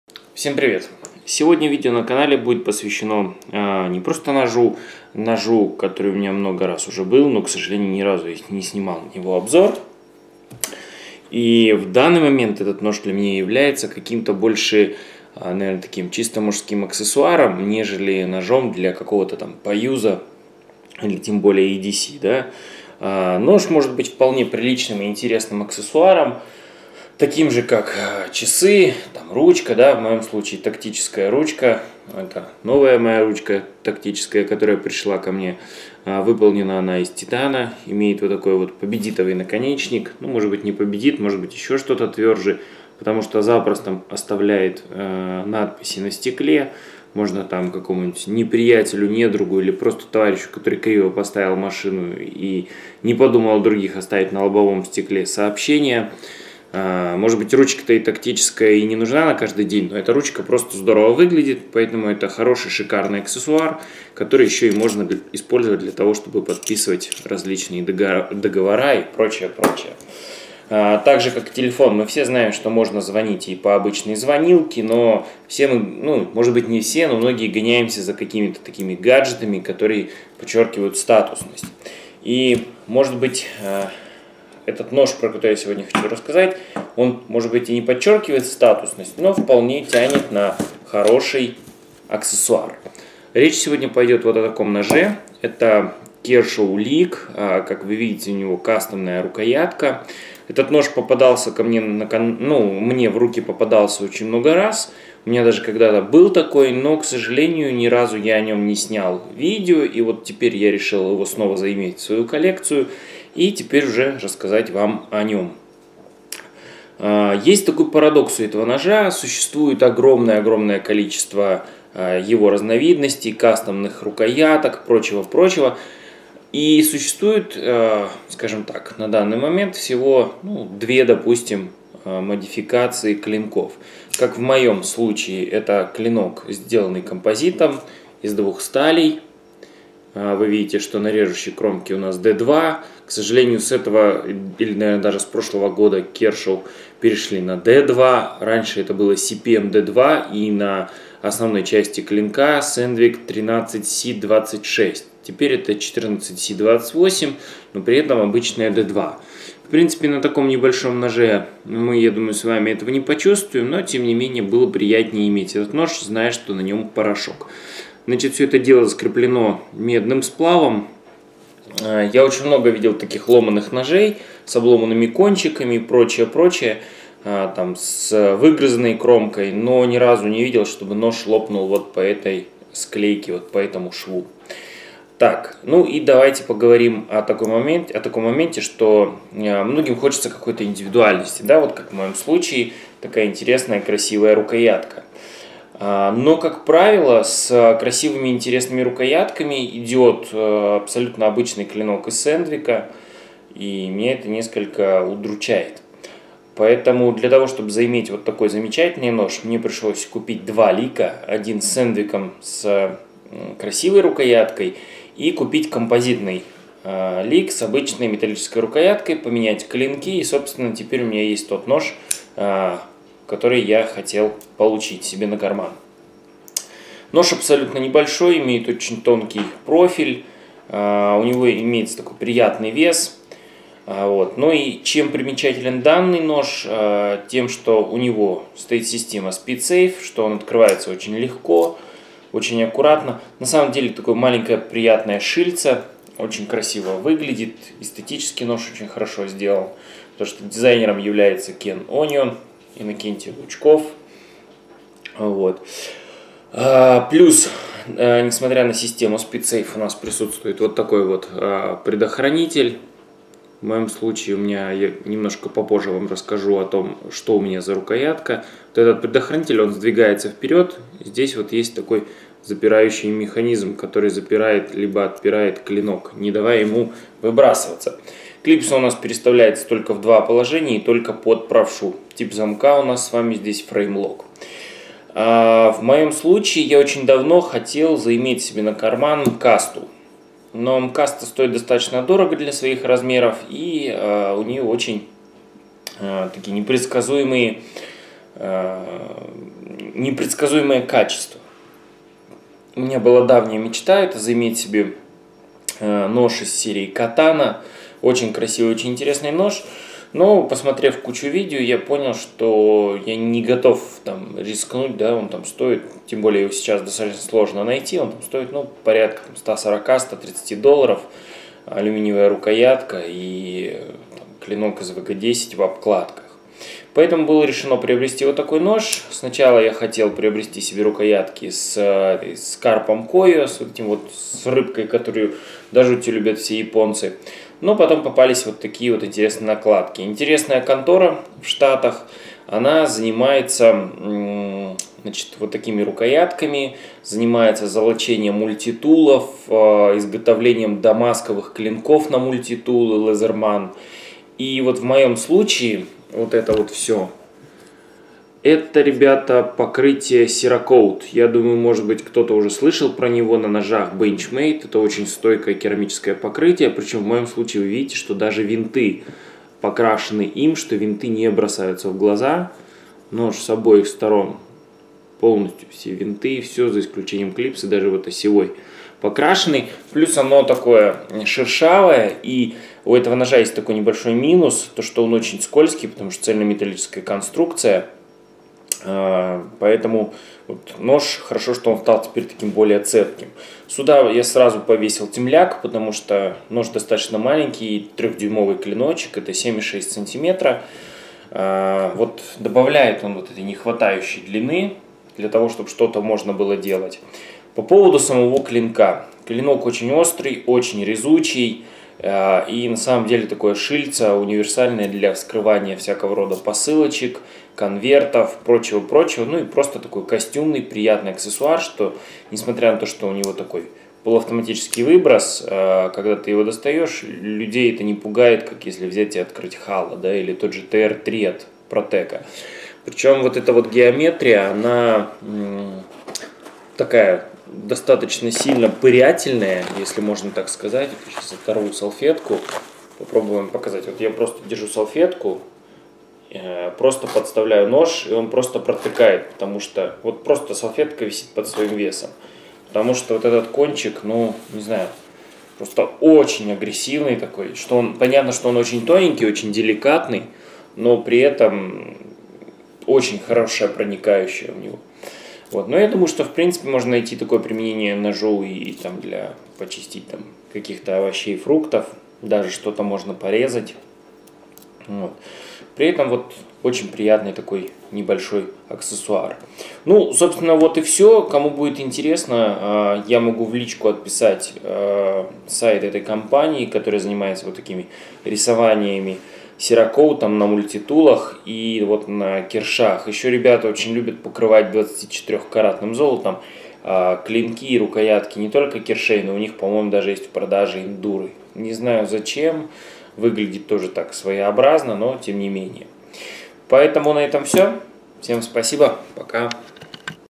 обзор Kershaw Leek Custom Composite
Отличный джентельменский нож, изготовленный с использованием интересных технологий.